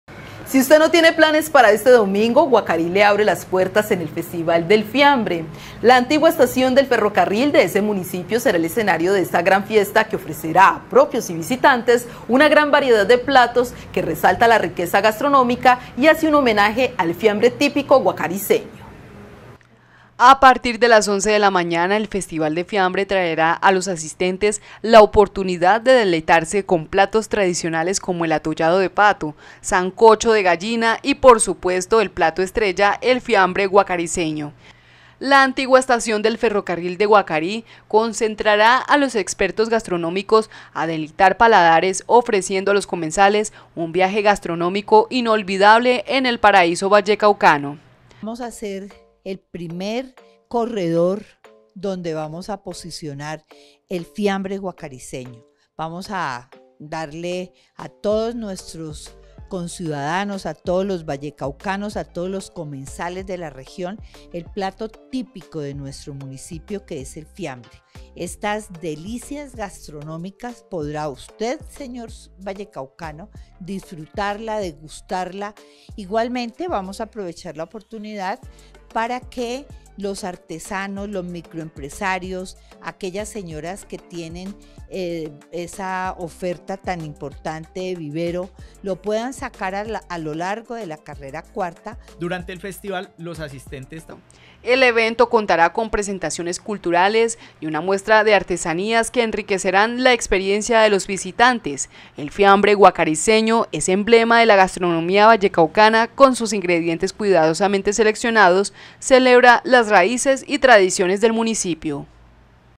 Festival del Fiambre Guacariceño
Si usted no tiene plan para este domingo, Guacarí le abre sus puertas en el festival del Fiambre. La antigua estación del ferrocarril del municipio será el escenario de esta gran fiesta que ofrecerá a los propios y visitantes una gran variedad de platos que resalta la riqueza gastronómica y hace un homenaje al fiambre típico guacariceños.